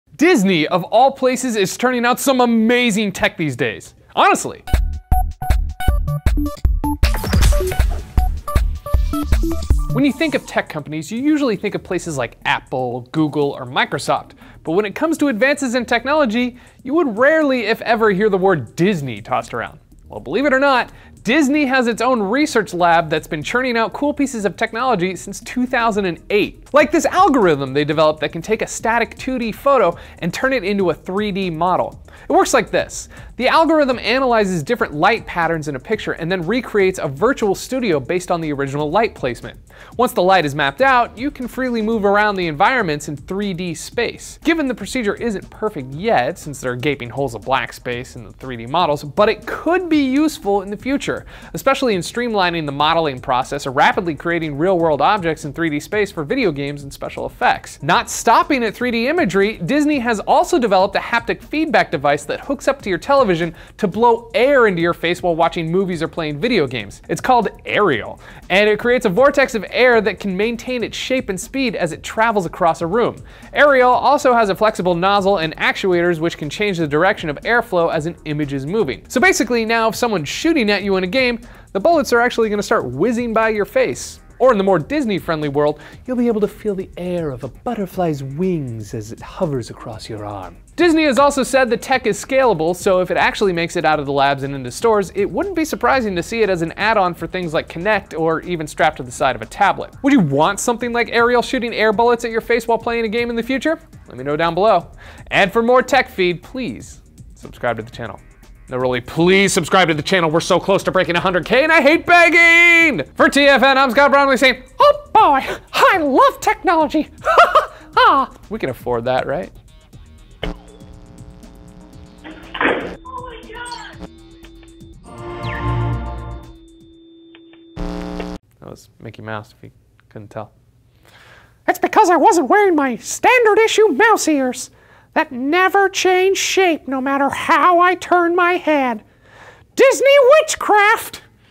Disney Blows You Away... Literally!
Disney is researching and developing new technology centered on users' and creators' perceptive feedback. Scott looks into Disney's developments in turning 2D photos into 3D models, and haptic feedback that shoots air in your face!

Is this immersion what we need for games and movies?

Host: Scott Bromley
Producer: Lauren Rudser
Associate Producers: Todd Boudreaux, Chastity Vicencio
Editor: Frank Knight